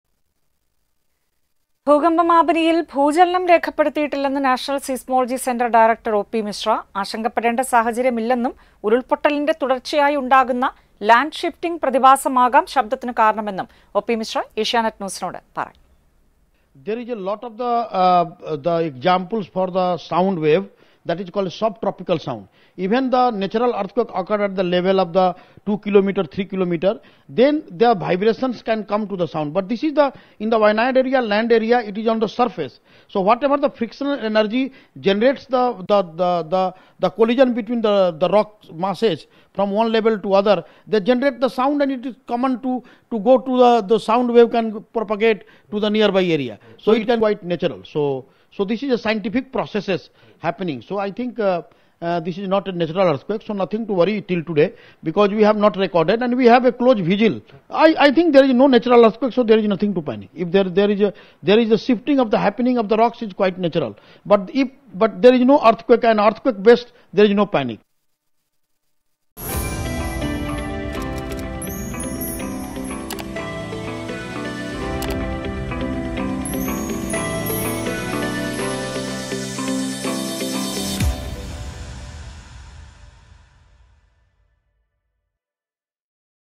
ഭൂചലനം രേഖപ്പെടുത്തിയിട്ടില്ലെന്ന് സീസ്മോളജി സെന്റർ ഡയറക്ടർ
'ഭൂചലനം രേഖപ്പെടുത്തിയിട്ടില്ല. ആശങ്ക വേണ്ട', ശബ്ദത്തിന് കാരണം ഉരുൾപൊട്ടലിന്റെ തുടർച്ചയായുണ്ടാകുന്ന പ്രതിഭാസമാകാമെന്ന് നാഷണൽ സീസ്മോളജി സെന്റർ ഡയറക്ടർ